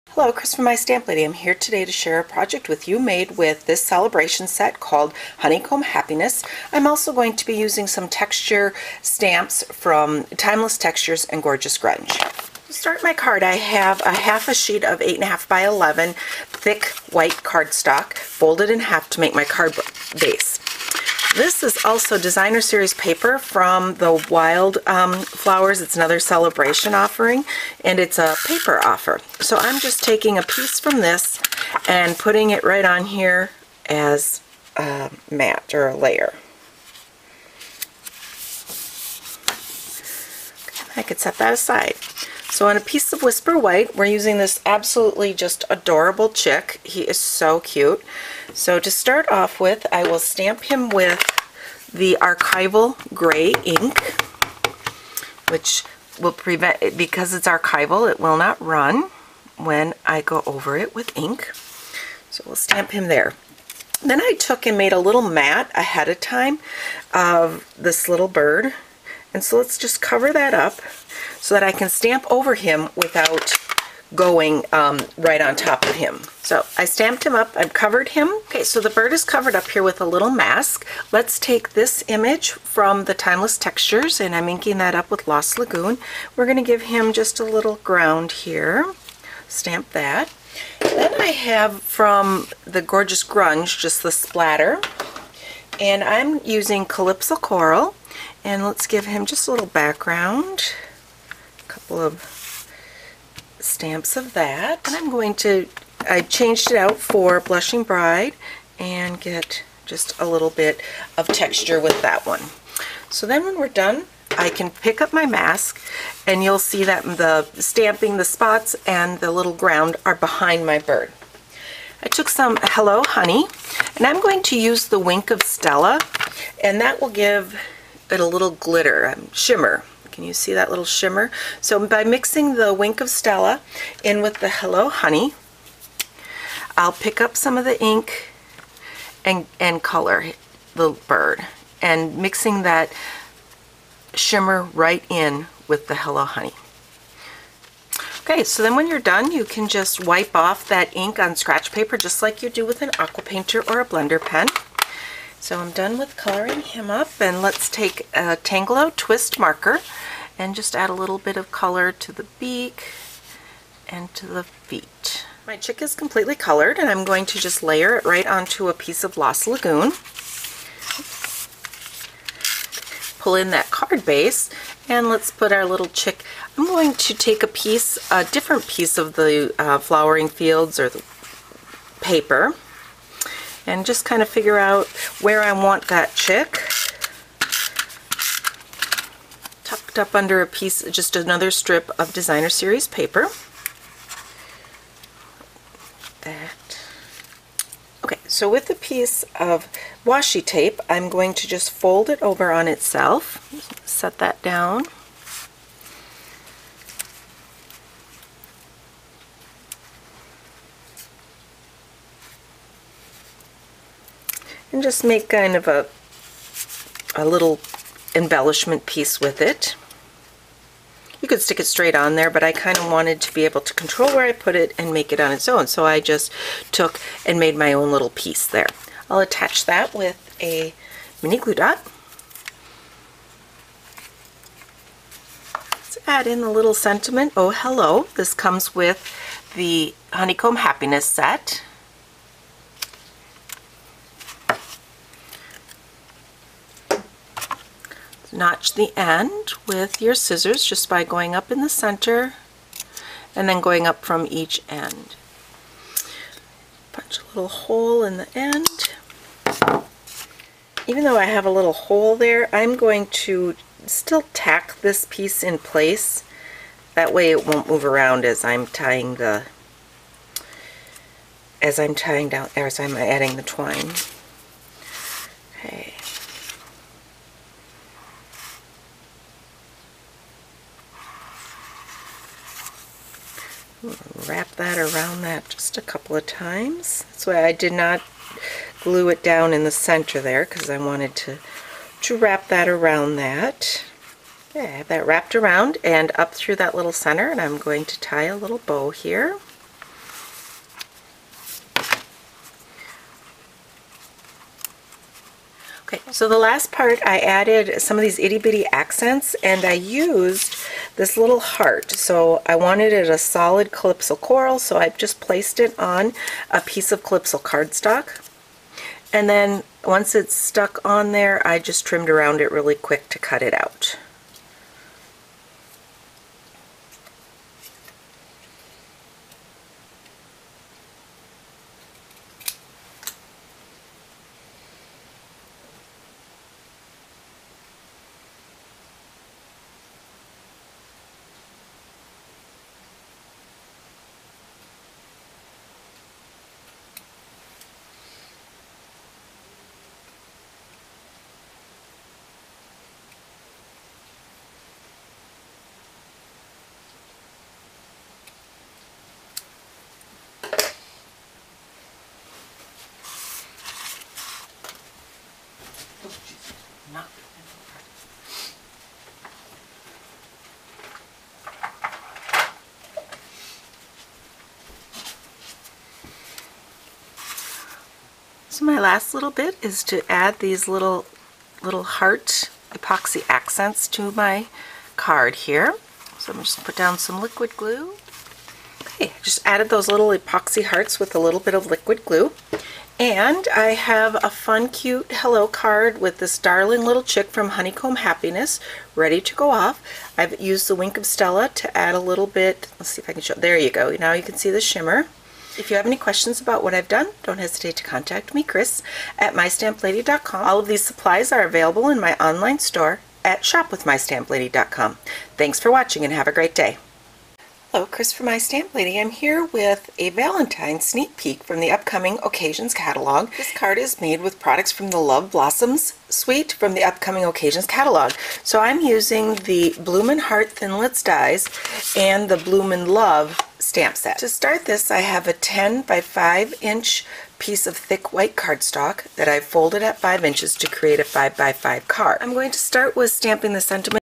Masking With Stampin' Up!'s Honeycomb Happiness
Masking With The Honeycomb Happiness Stamp Set@ http In this crafting tutorial video see how to mask an image to create the background and use this adorable chick from the Stampin' Up! Honeycomb Happiness stamp set.